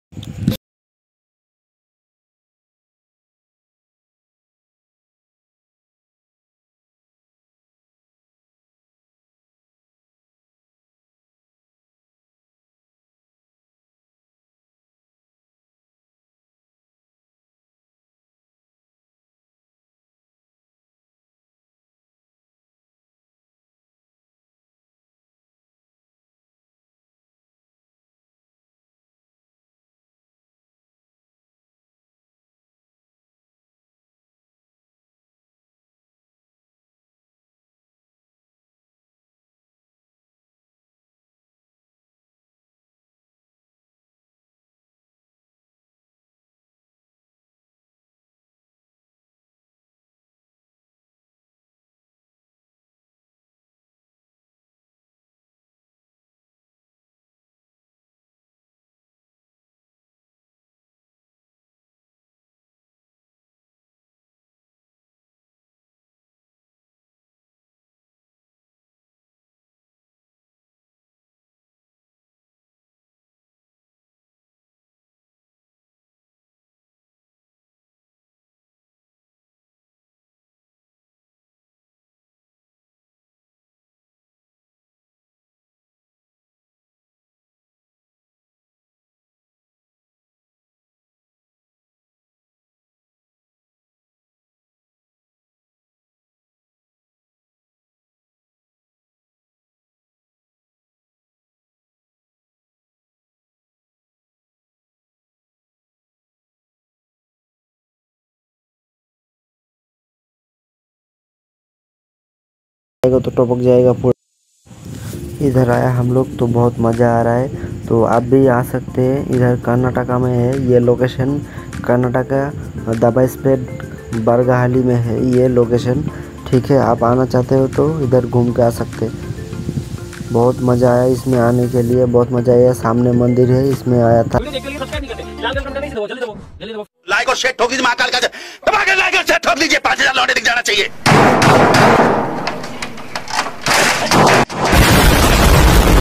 Today Long Table |आज हम बहुत दूर मंदिर देखने जा रहे हैं पहाड़ पर।minivlogger
Hey guys, we are going on a very long journey today. We are going to visit a mountain temple in Karnataka as it is 15th August.